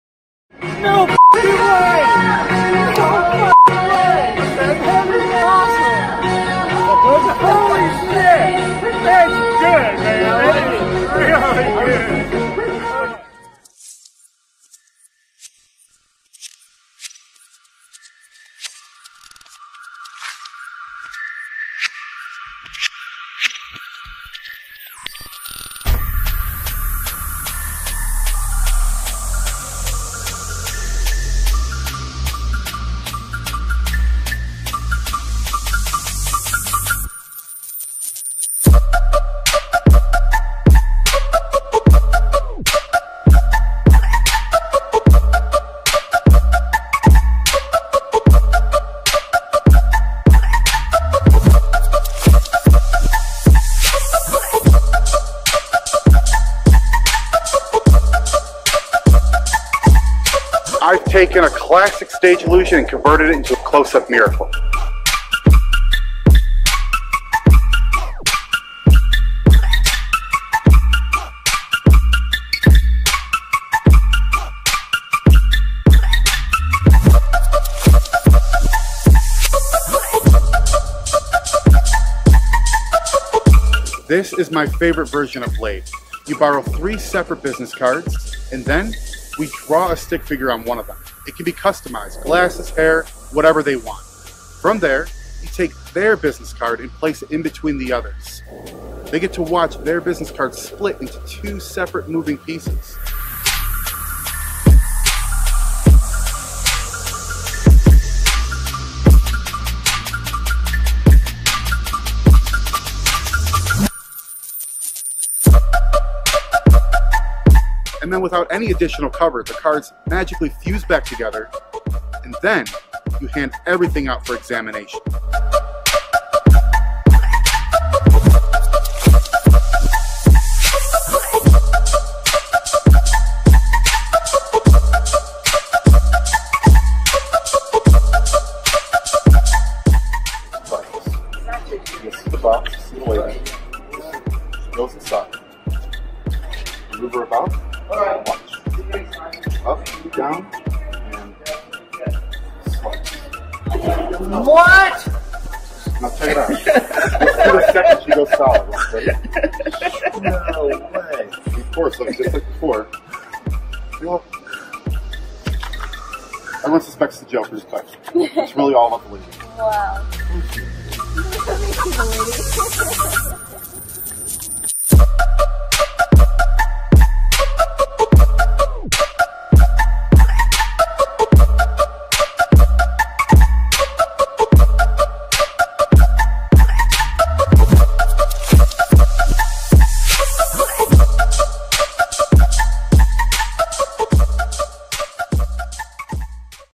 Blade by Nicholas Lawrence
Available from Lightning Magic Shop

From the creative mind of Nicholas Lawrence comes Blade: a classic stage illusion turned close-up!

An homage to the famous Sawing a Woman in Half illusion, Blade allows you to mysteriously split an ordinary card into two separate moving pieces right in front of your spectator's eyes! After the card is split, you effortlessly fuse the pieces back together without any additional cover. The best part? You can immediately hand everything out for examination!

- Blade arrives ready to perform right out of the box.
- Each gimmick is handmade by Nicholas Lawrence and the Abstract Effects team.
- Immediately hand everything out for examination.
- Each package comes with a fully built gimmick as well as additional materials for customization.